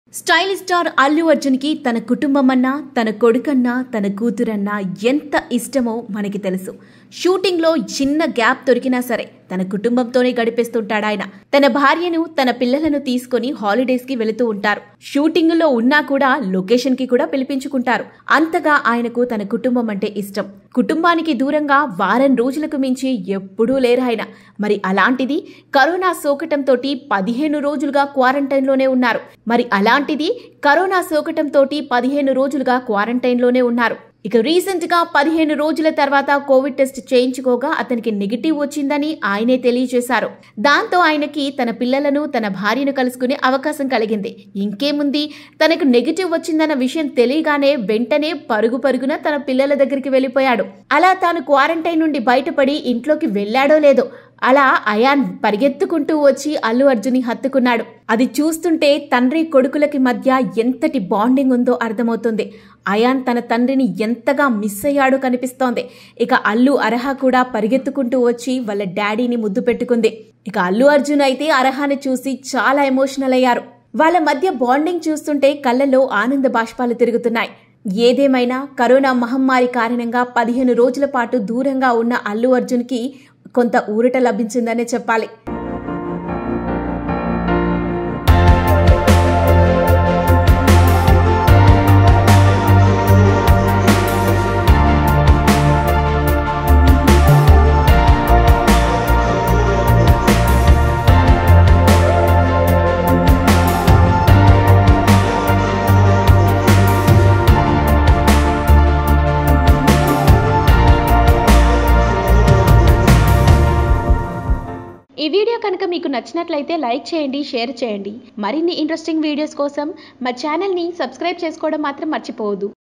Allu Arjun Gets Emotional with Arha & Ayaan | Allu Arjun Meets His Family After 15Days Of Quarantine
Watch, Allu Arjun Gets Emotional with Arha & Ayaan | Allu Arjun Meets His Family After 15Days Of Quarantine